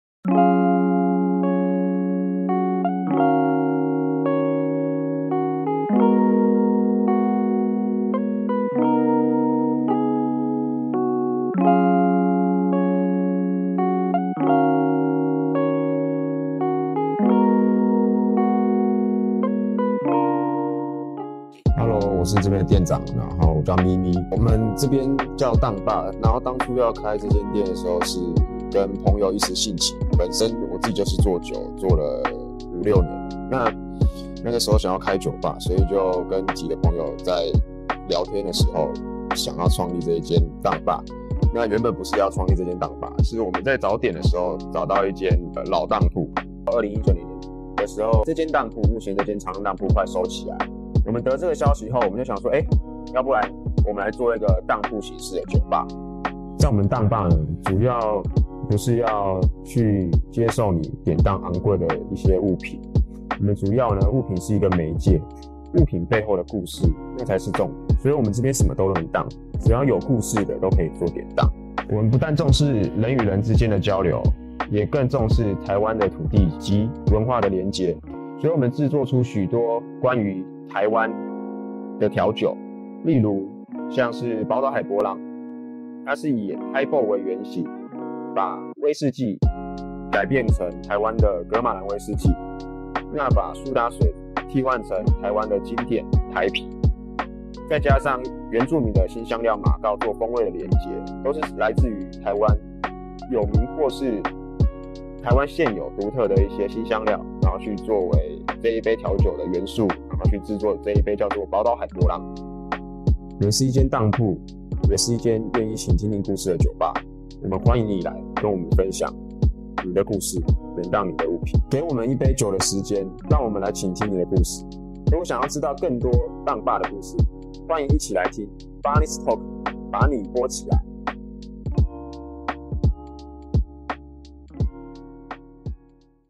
[影片 Podcast ] 典當回憶，黃湯下肚｜當吧Pawn Bar [ Spotify 限定]
最近 Spotify 開啟 Videocast 功能，想要體驗有影片的 Podcast 就快到 Spotify 上收聽吧～（其他平台只能聽到聲音）

「有酒就有故事，有故事就能典當。」酒吧喝酒不稀奇，若還能將人們視為珍寶的回憶、舊物典當折抵酒費，那就是一項相當有趣的體驗。回憶的背後往往藏著主人心中最柔軟、難以碰觸的過往和曾經，也許是一段刻骨銘心的感情、也許是一段兒時記憶，亦或是旅行時的風景，在「 當吧 PAWN BAR」，每段故事都能被好好安放、好好傾聽、好好收藏，或許等待下一個主人，展開新的面貌和旅程。